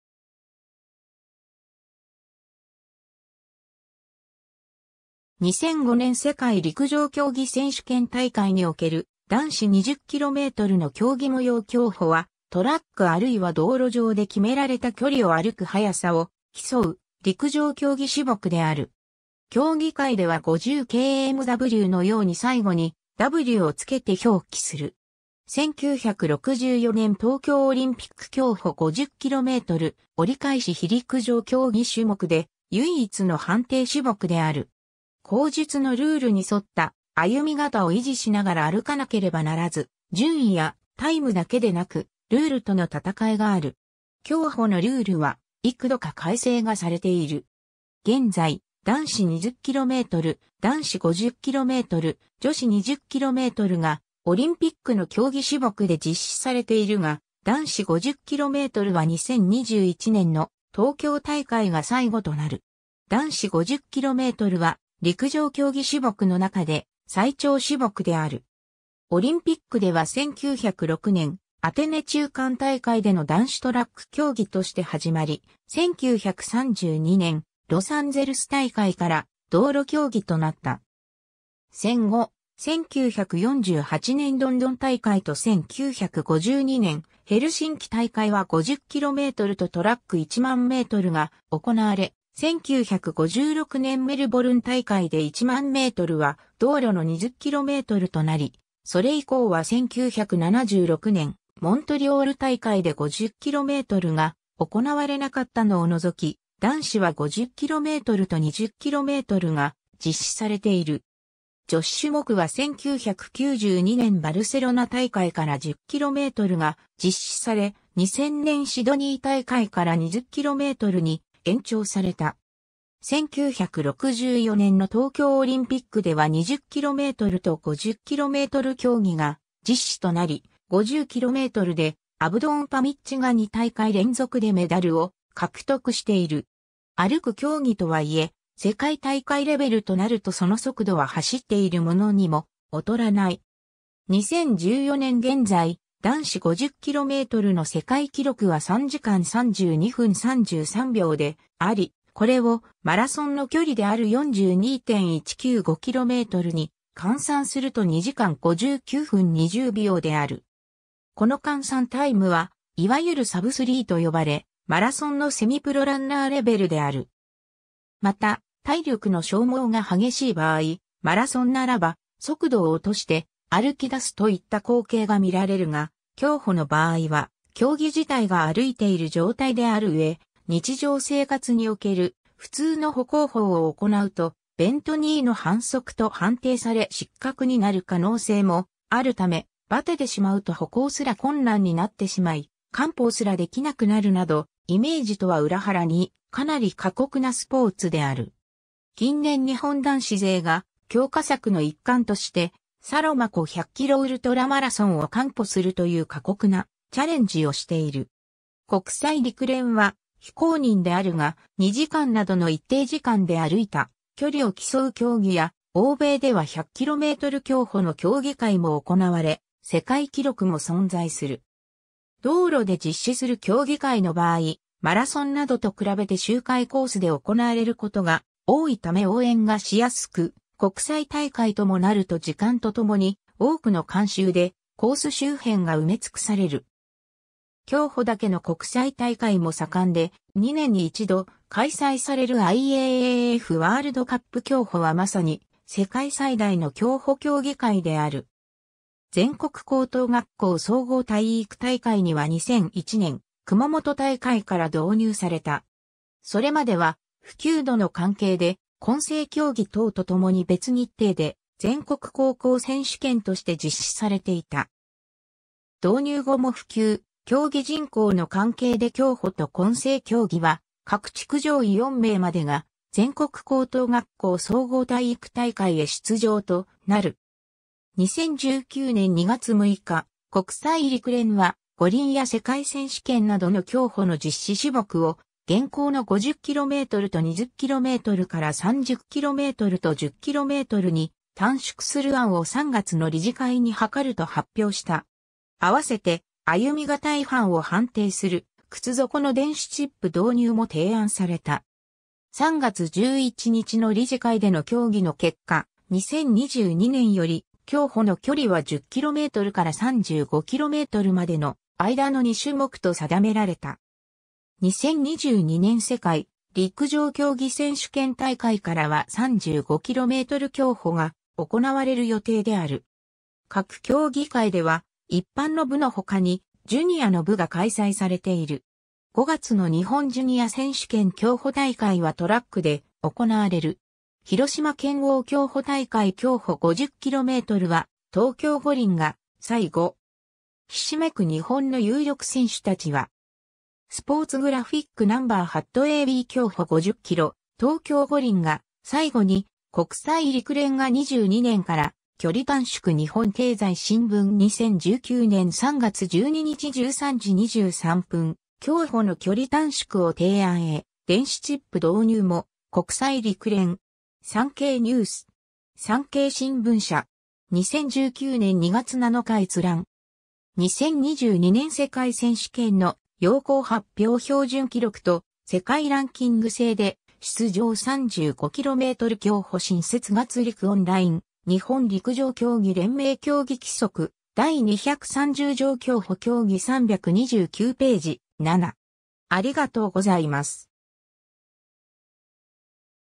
競歩
2005年世界陸上競技選手権大会における男子20kmの競技模様 競歩（きょうほ）は、トラックあるいは道路上で決められた距離を歩く速さを競う陸上競技種目である。
競技会では50kmWのように最後にW (walk) を付けて表記する。
 1964年東京オリンピック競歩50km折り返し碑（府中市 (東京都)） 陸上競技種目で唯一の判定種目である。
後述のルールに沿った歩形（フォーム）を維持しながら歩かなければならず、順位やタイムだけでなくルール（失格）との戦いがある。
競歩のルールは幾度か改正がされている。
 現在、男子20km・男子50km・女子20kmがオリンピックの競技種目で実施されているが、男子50kmは2021年の東京大会が最後となる。
男子50kmは陸上競技種目の中で最長種目である。
オリンピックでは1906年アテネ中間大会での男子トラック競技として始まり､1932年ロサンゼルス大会から道路競技となった(50km)。
戦後、1948年ロンドン大会と1952年ヘルシンキ大会は50kmとトラック10000mが行なわれ、1956年メルボルン大会で10000mは道路の20kmとなり、それ以降は1976年モントリオール大会で50kmが行なわれなかったのを除き、男子は50kmと20kmが実施されている。
女子種目は1992年バルセロナ大会から10kmが実施され、2000年シドニー大会から20kmに延長された。
1964年の東京オリンピックでは20kmと50km競技が実施となり、50㎞でアブドン・パミッチが2大会連続でメダルを獲得している。
 歩く競技とはいえ、世界大会レベルとなるとその速度は走っているものにも劣らない。
2014年現在、男子50kmの世界記録は3時間32分33秒であり、これをマラソンの距離である42.195kmに換算すると2時間59分20秒である。
この換算タイムはいわゆるサブスリーと呼ばれ、マラソンのセミプロランナーレベルである。
また、体力の消耗が激しい場合、マラソンならば速度を落として「歩きだす」といった光景が見られるが、競歩の場合は（スピードは速いが）競技自体が歩いている状態であるうえ、日常生活における普通の歩行法を行うと「ベント・ニー」の反則（後述）と判定され失格になる可能性もあるため、バテてしまうと歩行すら困難になってしまい完歩すら出来なくなるなど、イメージとは裏腹にかなり過酷なスポーツである。
近年日本男子勢が強化策の一環としてサロマ湖100キロウルトラマラソンを完歩するという過酷なチャレンジをしている。
国際陸連は非公認であるが、2時間などの一定時間で歩いた距離を競う競技や、欧米では100km競歩の競技会も行なわれ、世界記録も存在する。
 道路で実施する競技会の場合、マラソンなどと比べて周回コースで行なわれることが多いため応援がしやすく、国際大会ともなると時間とともに多くの観衆でコース周辺が埋め尽くされる（マラソンと同様、コース上での観戦は入場料はかからない）。
競歩だけの国際大会も盛んで、2年に一度開催されるIAAFワールドカップ競歩はまさに世界最大の競歩競技会である。
 全国高等学校総合体育大会には2001年熊本大会から導入された。
それまでは普及度の関係で混成競技等と共に別日程（3週間遅れくらい）で全国高校選手権として実施されていた。
導入後も普及・競技人口の関係で競歩と混成競技は各地区上位4名まで（2009年までは3名）が全国高等学校総合体育大会へ出場となる（他種目は上位6名まで）。
 2019年2月6日、国際陸連は、五輪や世界選手権などの競歩の実施種目を、現行の50kmと20kmから30kmと10kmに短縮する案を3月の理事会に諮ると発表した。
合わせて、歩型違反を判定する靴底の電子チップ導入も提案された。
3月11日の理事会での協議の結果、2022年より競歩の距離は10kmから35kmまでの間の2種目と定められた。
2022年世界陸上競技選手権大会からは35km競歩が行われる予定である。
 各競技会では一般の部のほかに、ジュニア（高校生、中学生）の部が開催されている。
5月の日本ジュニア選手権競歩大会はトラックで行われる。
 ・ひろしま県央競歩大会 ^ 競歩50kmは、東京五輪が「最後」。
ひしめく日本の有力選手たちは……。
 - Sports Graphic Number ^ a b 競歩50キロ、東京五輪が最後に　国際陸連が22年から距離短縮 - 日本経済新聞 2019/3/12 13:23 (...